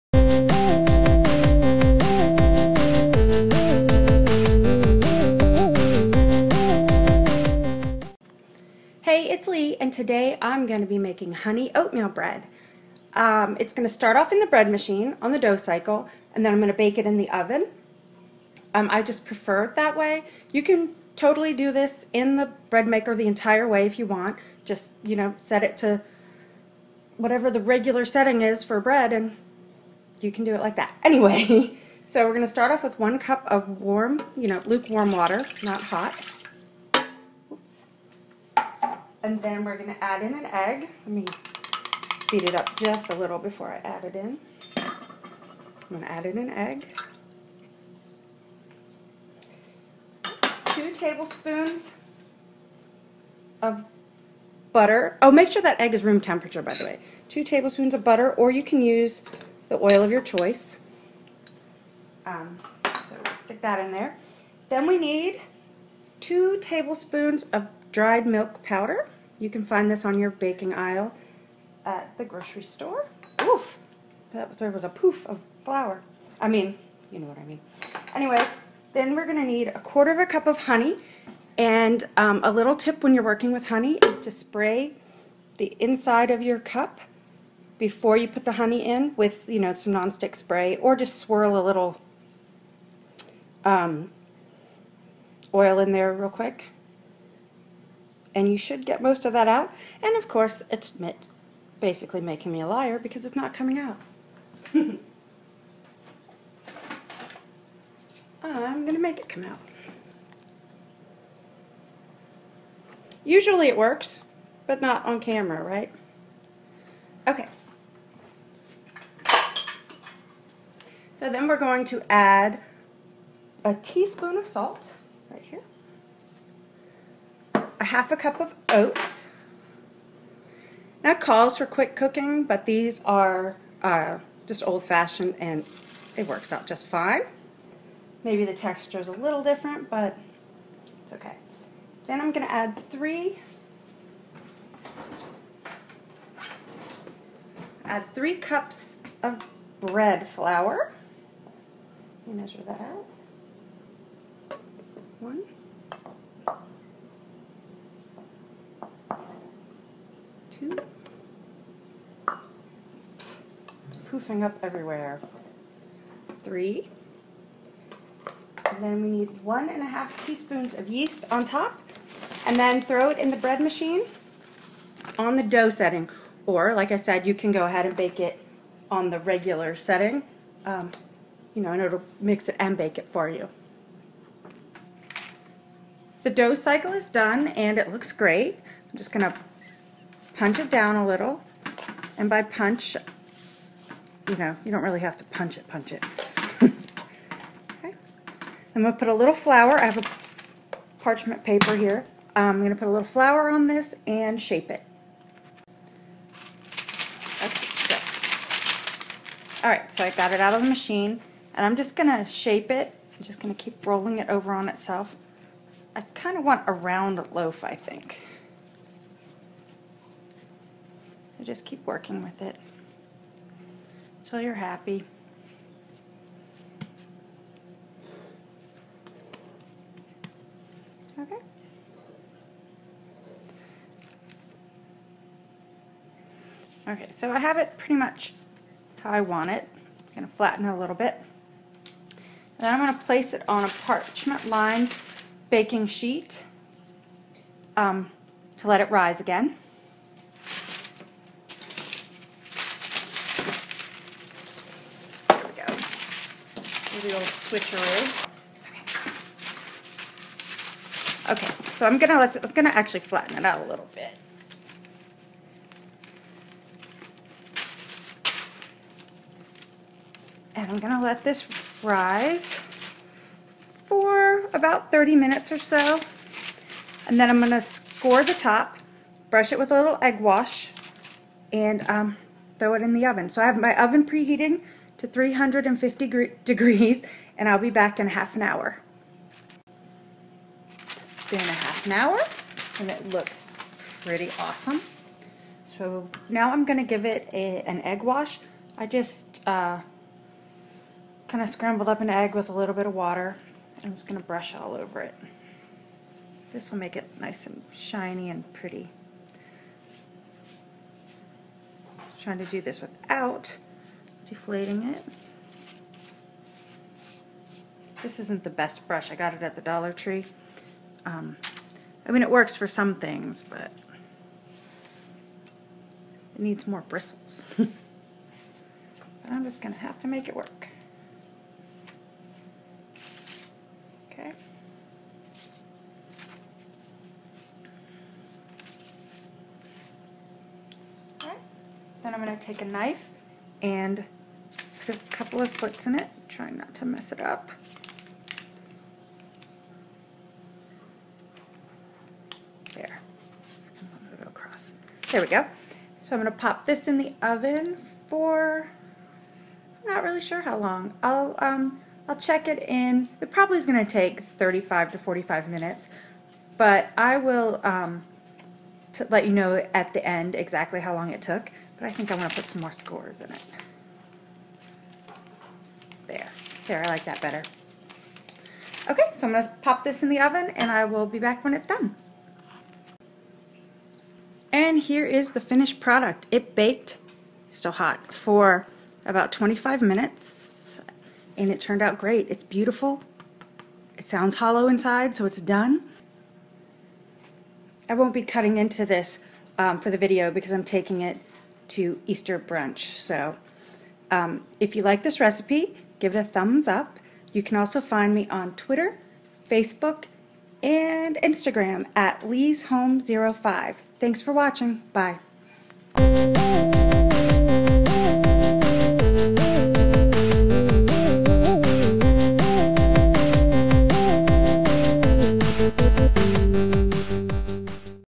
HONEY OATMEAL BREAD Recipe using the Bread Machine
This recipe for Honey Oatmeal Bread is a family favorite!  It's so easy to make especially since the dough is made in a bread machine.  Try it out and let me know what you think.

Honey Oatmeal Bread
1 cup warm water
1 egg
2 T. butter (I use Canola or Olive Oil)
2 T. dry milk
1/4 cup honey
1 t. salt
1/2 cup quick oats
3 cups flour (I use bread flour)
1 1/2 t. yeast

Usually you will need to watch the dough after it has been kneading for about 5 minutes and add either water or flour the make the dough ball smooth.  Add only about 1 t. of water at the time or same amount of flour.  The more you make the bread the more you will know what and when to do it.
You can either make rolls with the dough or just allow it to bake for your loaf of bread.

Camera - Canon PowerShot SX530
Bread Machine
Instant Pot Duo Mini
Kitchen Aid Mixer (except mine is cream)
These are a few of my favorite things